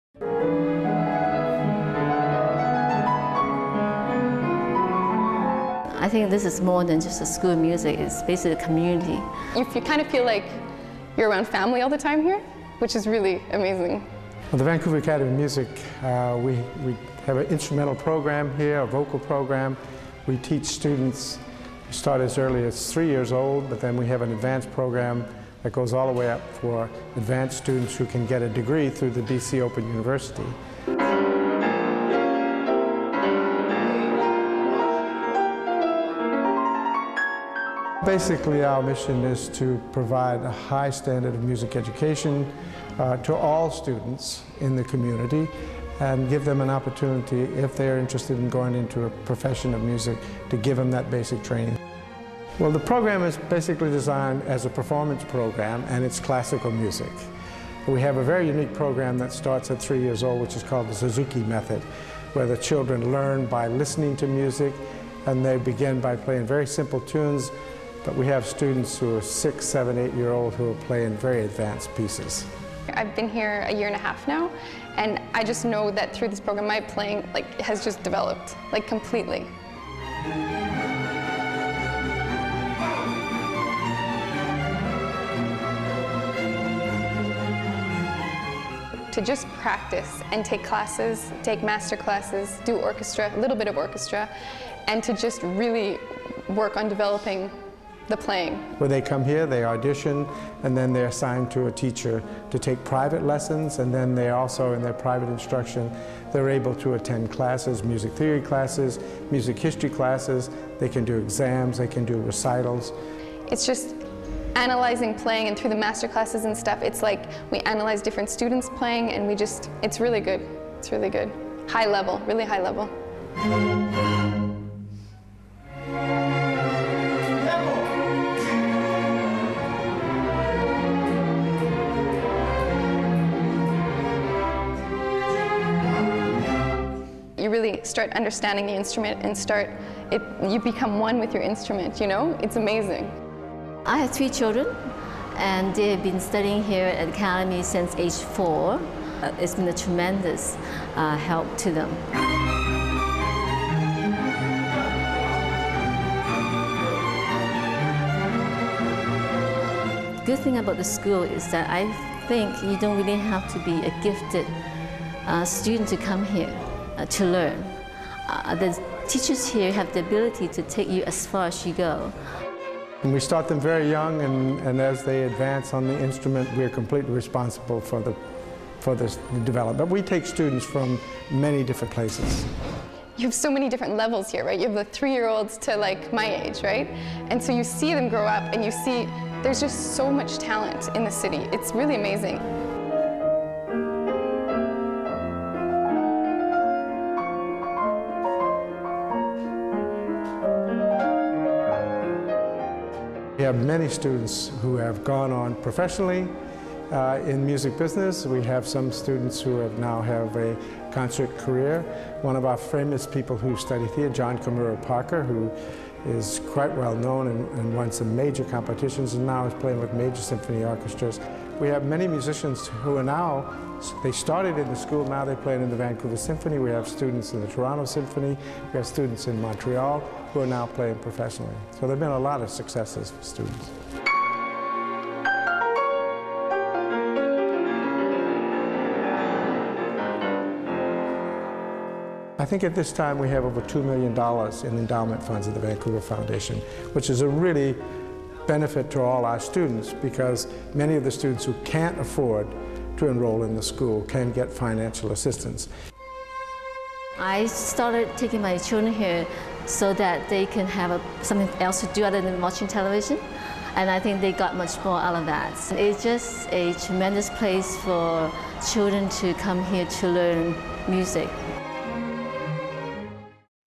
Helping young musicians grow - Vancouver Academy of Music
The Academy is more than just a school of music. It's a community where students can grow as musicians.

They offer classical classes for all levels from three-year olds learning the Suzuki method to aspiring professional musicians. Their mission is to provide a high standard of music education to all students in the community. Many members of the Vancouver Symphony Orchestra teach at the Academy.

They have approximately $2 million in their endowment fund at Vancouver Foundation, which they use to offer financial assistance to students and help them realize their full potential.

(Originally aired: 2005)